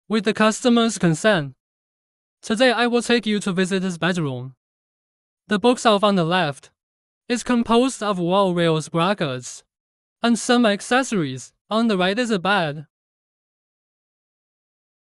What does a great bedroom look like?
Some time ago, the decoration of the client's home was completed. With the client's permission, we display it. You can see that the bedroom is very simple and fully functional. Very suitable for studying and working. The bookshelf on the left is our result and the client loved it.